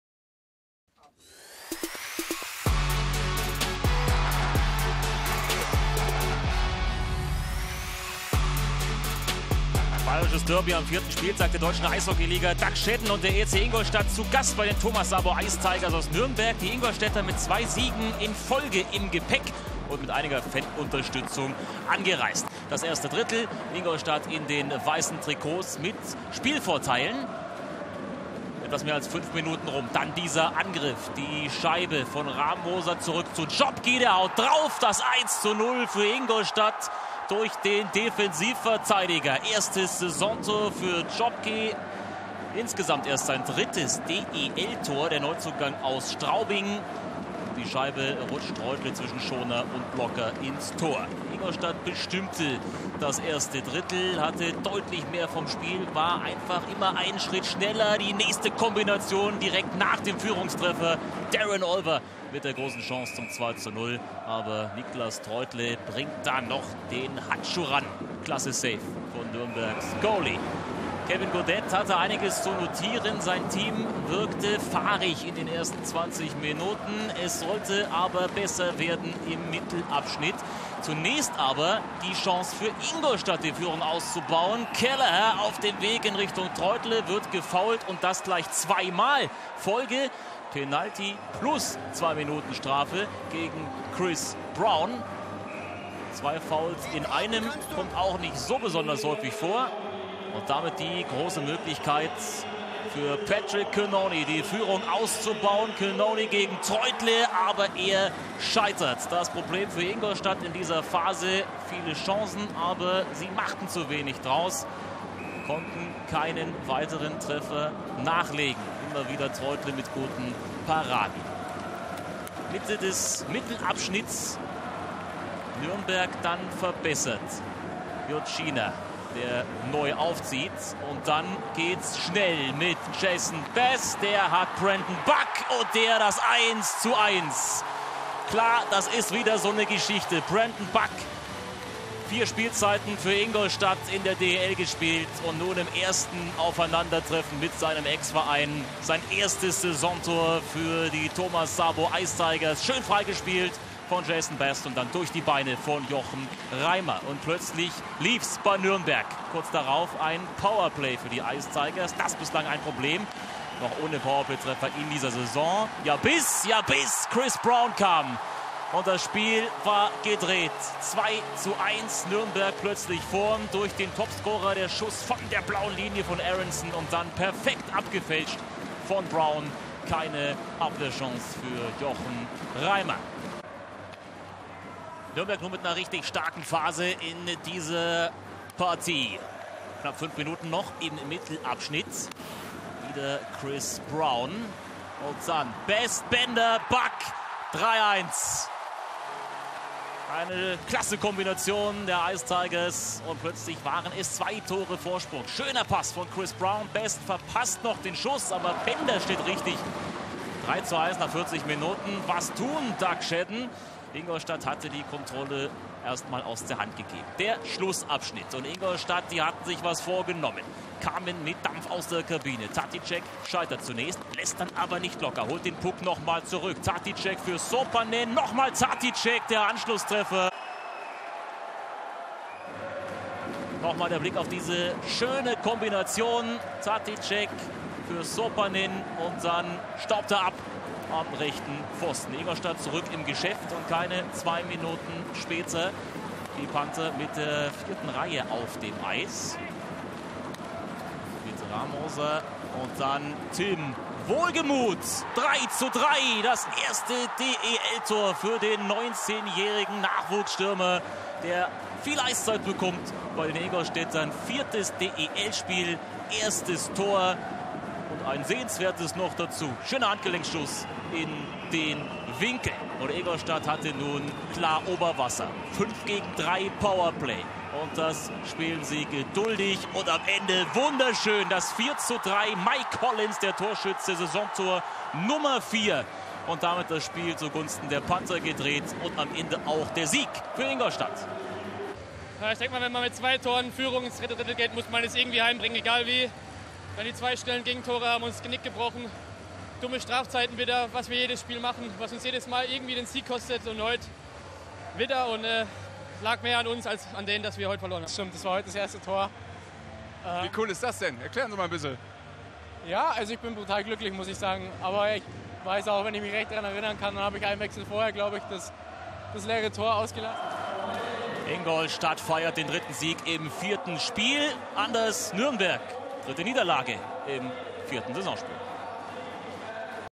Thomas Sabo Ice Tigers – ERC Ingolstadt | 4. Spieltag, 18/19 | Telekom Sport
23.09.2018, 4. Spieltag der Saison 2018/2019. Aus dem bayerischen Derby zwischen den Thomas Sabo Ice Tigers und dem ERC Ingolstadt gehen die Gäste aus Ingolstadt nach einer abwechslungsreichen Begegnung mit 4:3 als Sieger hervor. Im Schlussdrittel drehte der ERC Ingolstadt die Partie durch Treffer von Petr Taticek, Tim Wohlgemuth und Michael Collins. Für die Thomas Sabo Ice Tigers war es die dritte Niederlage im vierten Spiel.

Aufstellungen:

Thomas Sabo Ice Tigers:
William Acton, Daniel Weiss, Leonhard Pföderl, Brett Festerling, Taylor Aronson, Niklas Treutle

ERC Ingolstadt:
Darin Olver, Petr Taticek, Vili Sopanen, Ville Koistinen, Maurice Edwards, Jochen Reimer